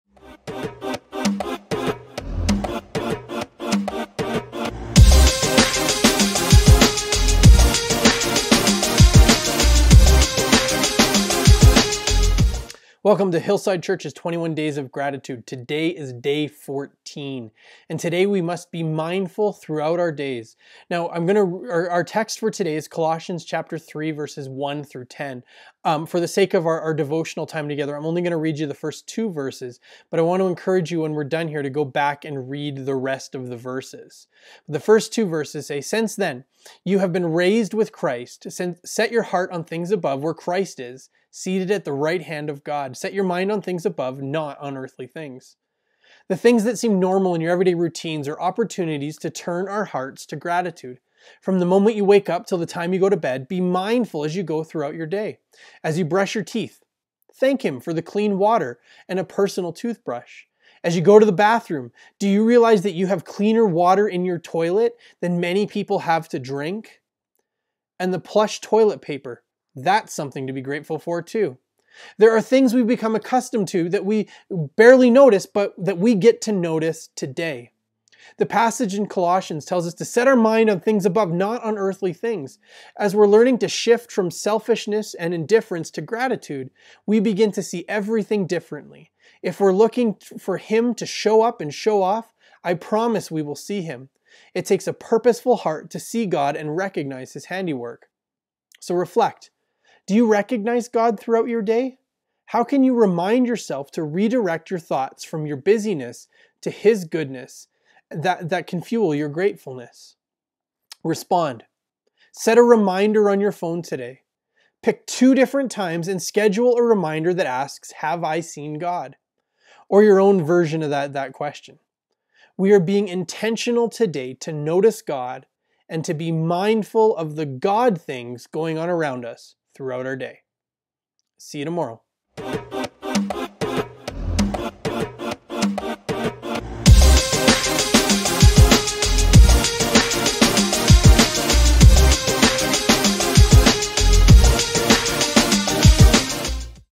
21 Days of Gratitude - Day 14
Day 14
We Must be Mindful Throughout Our Days
Colossians 3:1-10

What you need:
A Bible
A gratitude journal
A pen
An open heart to God working in your life